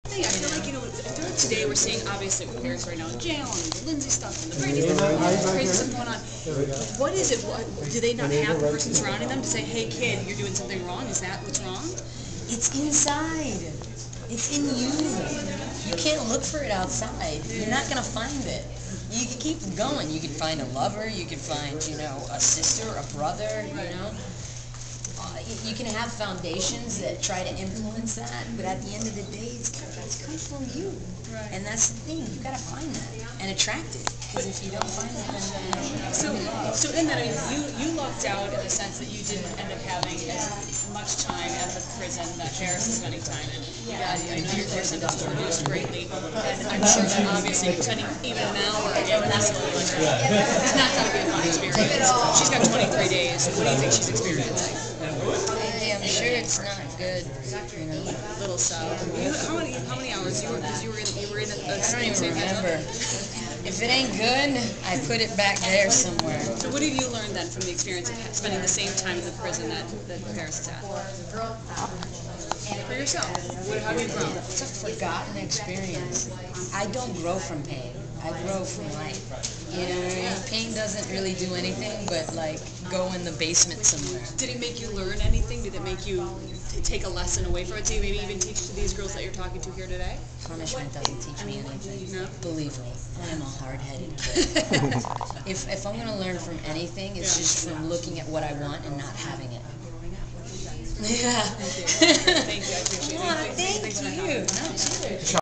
Michelle Rodriguez Interviewed By Samantha Harris
Michelle Rodriguez Interviewed By Samantha Harris at a special event in Beverly Hills.
Camera: Clinton H.Wallace/Photomundo International
Location: Beverly Hills Hotel